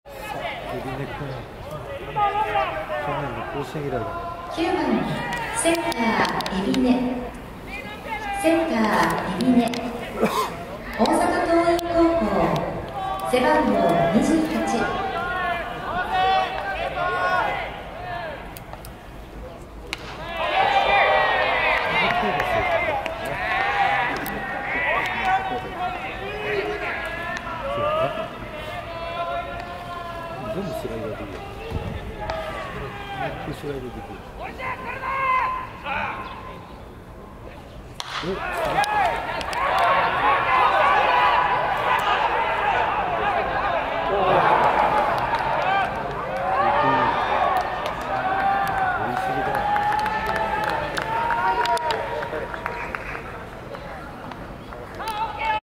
【海老根優大 JABA社会人野球 初ヒット】2023/3/7(火) 山田航大(アスミビルダーズ) vs 海老根優大(SUBARU) ショート内野安打
第77回 JABA東京スポニチ大会 2023/3/7(火) 明治神宮球場 アスミビルダーズ vs SUBARU 1回戦

2回裏（SUBARUの攻撃）
（先発投手）山田
1アウト 1塁 海老根 ショート内野安打

【勝投手】小澤
【敗投手】山田
小原(球)、松田(一)、廣田(二)、安部(三)

アスミビルダーズ vs SUBARU 試合経過-スポニチ創刊75周年記念 第77回JABA東京スポニチ大会 : 一球速報.com | OmyuTech